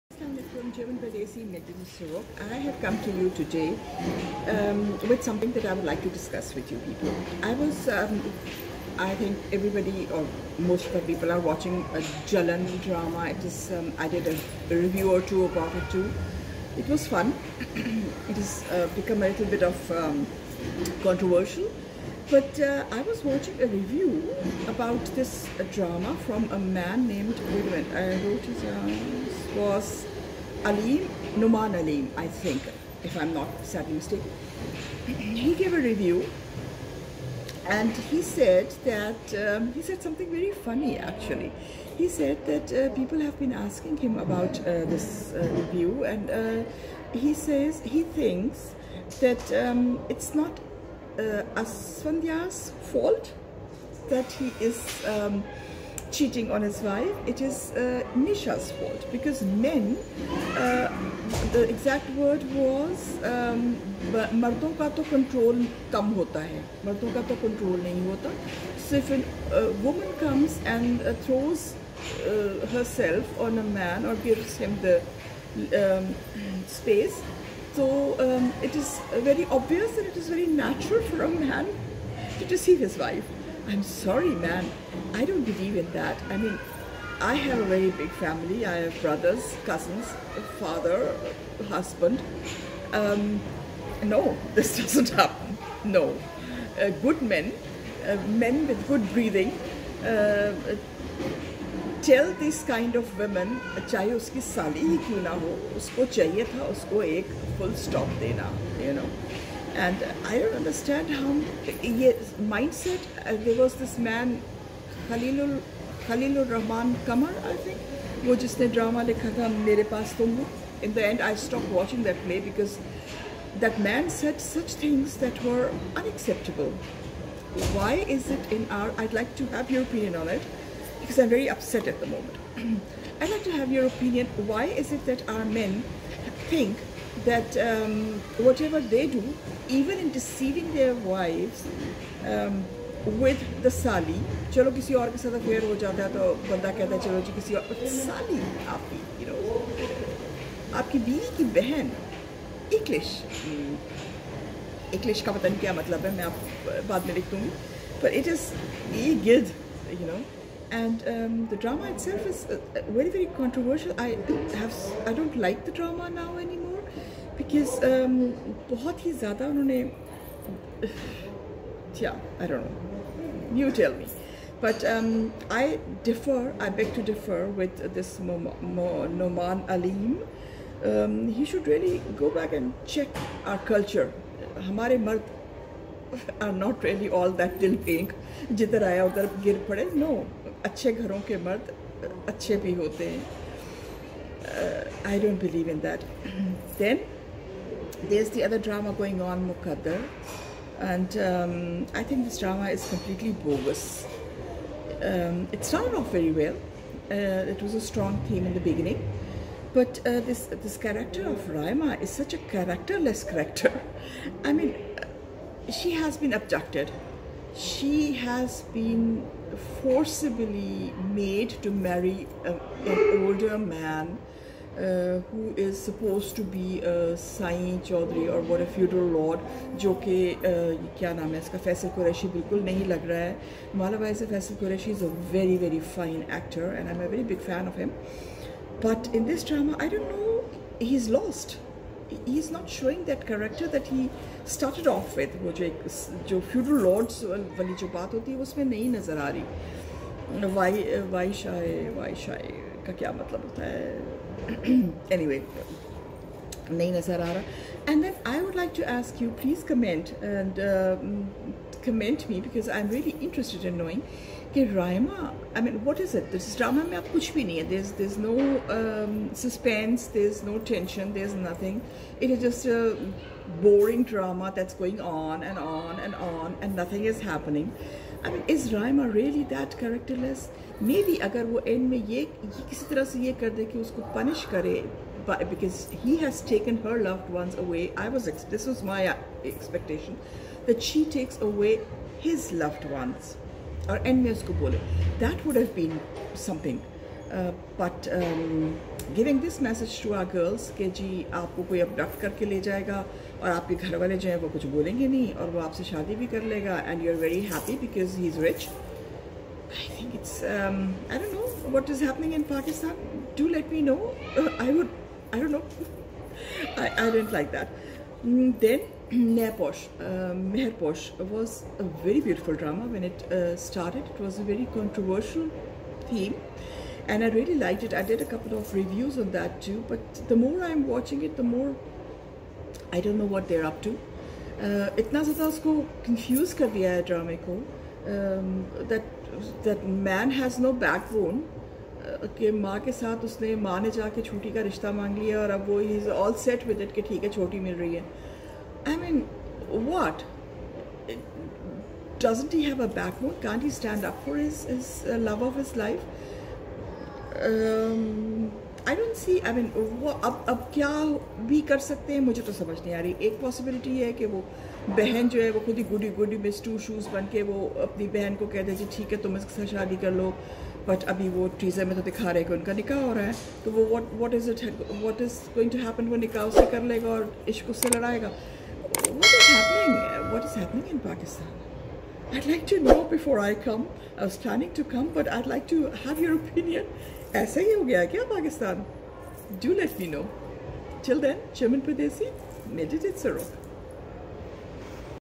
German Reaction | Jalan | Muqaddar | Mehrposh | Review for Week of September 14
German Pardesi reviews "Jalan" (ARY Digital), "Muqaddar" (Geo TV), & "Mehrposh" (Geo TV) for the week of September 14.